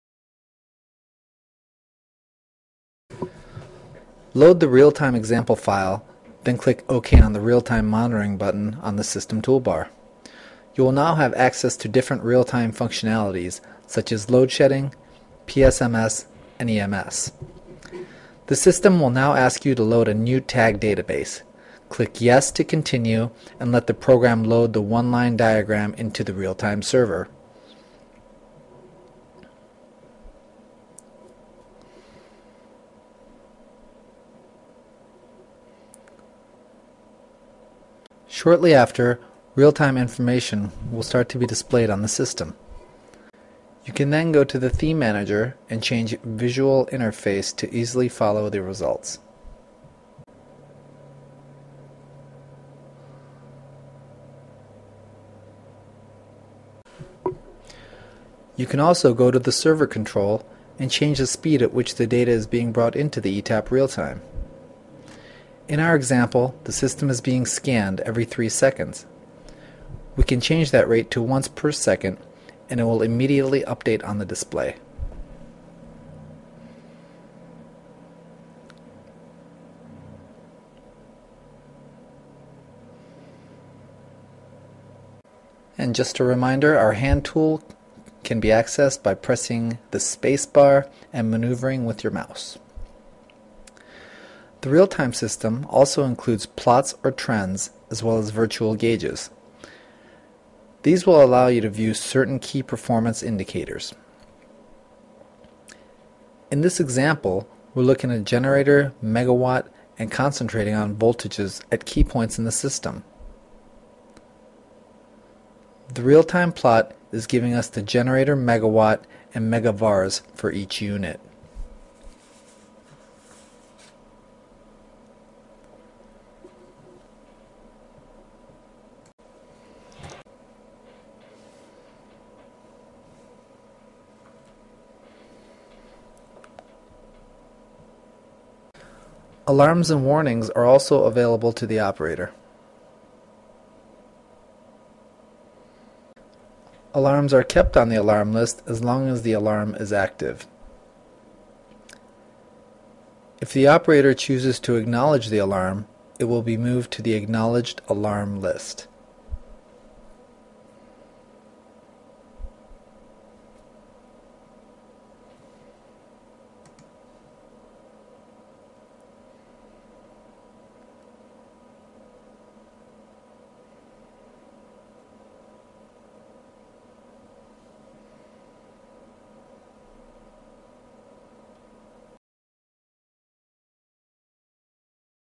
Real-Time Monitoring with ETAP Software
Connect ETAP to the actual system and retrieve data for visualization on an intelligent one-line diagram with this simple example.